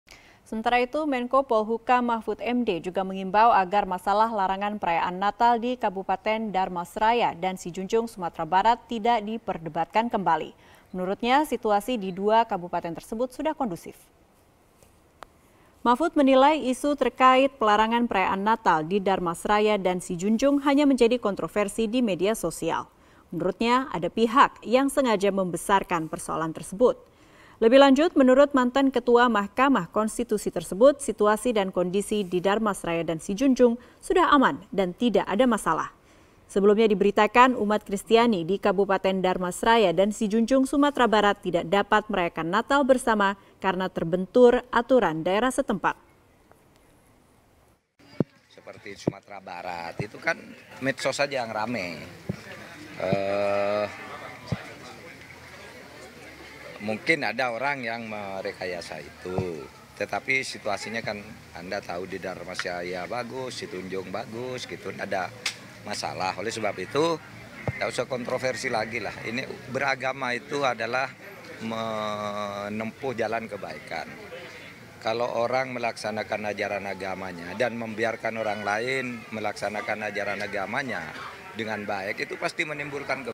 Mahfud MD Minta Masalah Perayaan Natal di Dharmasraya Disudahi
Menkopolhukam Mahfud Md, mengimbau agar masalah laranan perayaan natal di Kabupaten Dharmasraya dan Sijunjung, Sumatera Barat, tidak diperdebatkan kembali, menurutnya situasi di dua kabupaten tersebut sudah kondusif.